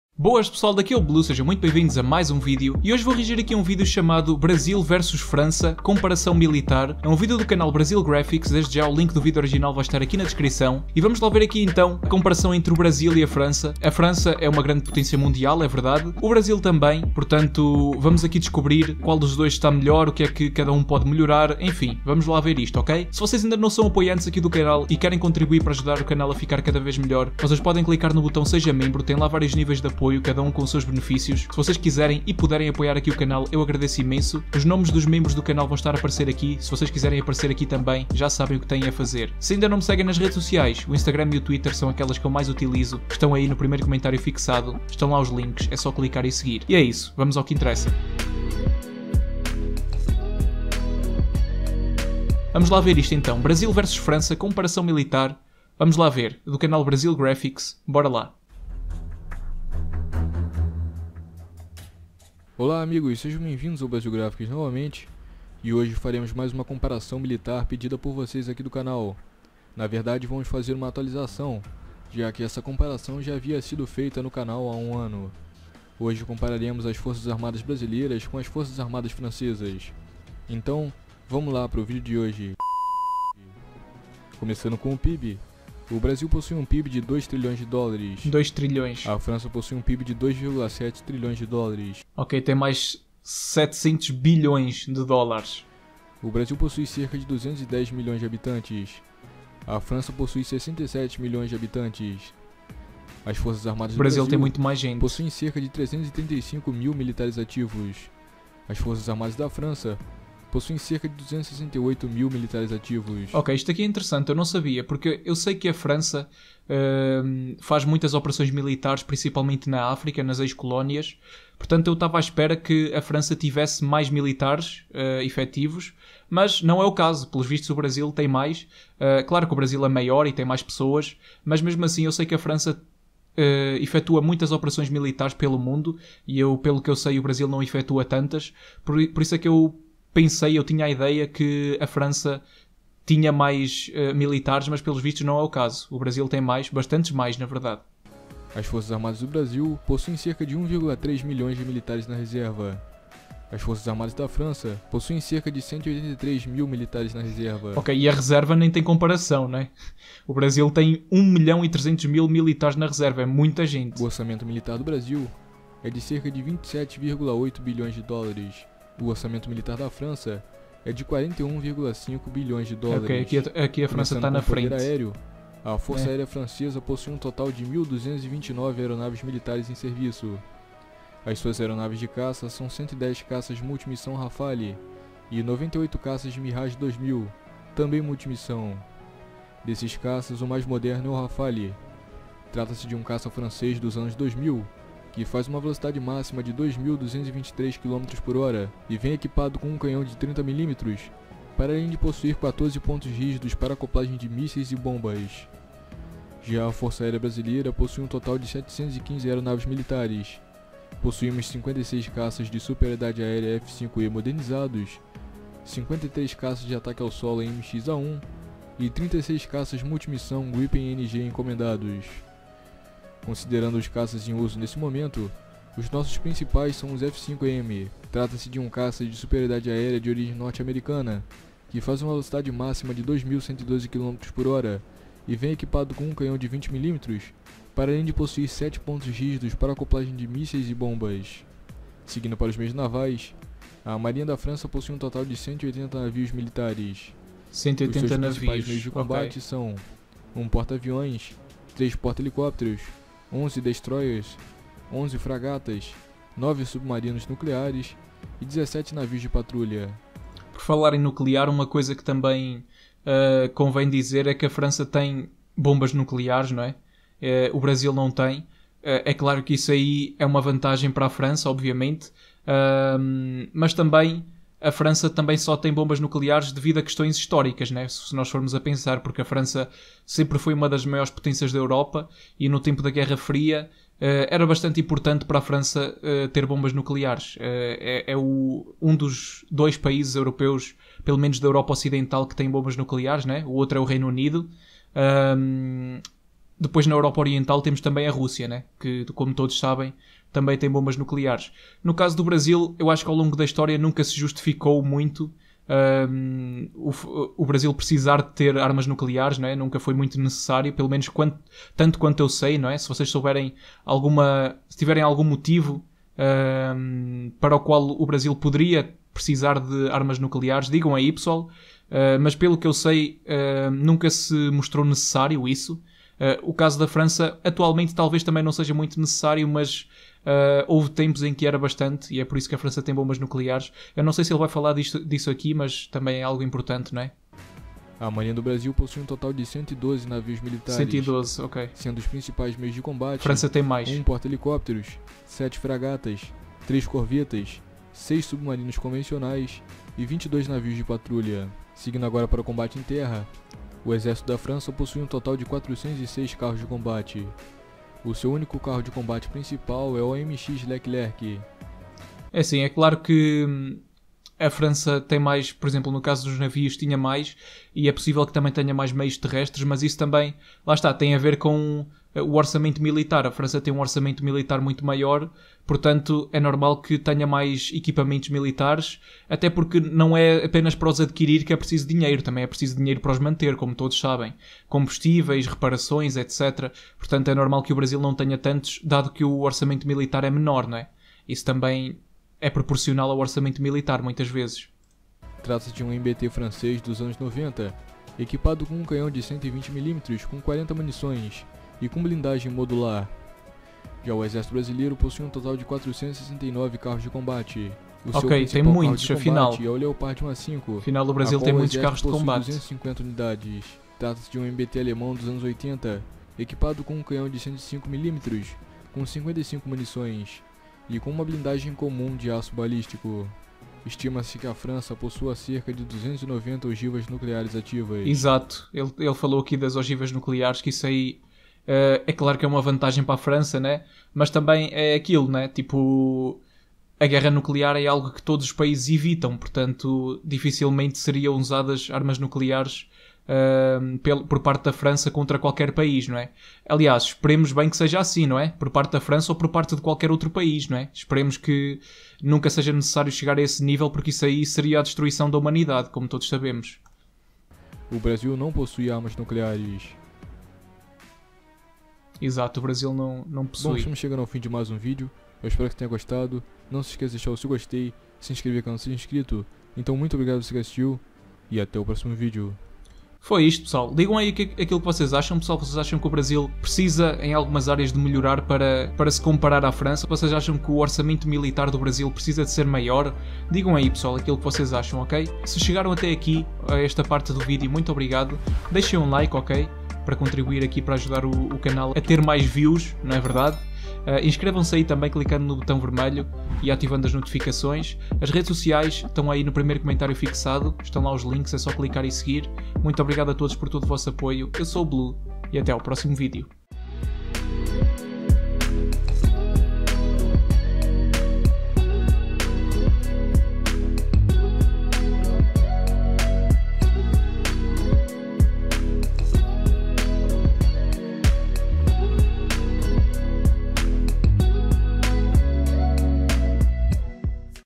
BRASIL X FRANÇA (Comparação Militar) [Português reage]
Servidor do Discord:
Se curtiste, não te esqueças do LIKE !
E inscreve-te, se ainda não o fizeste !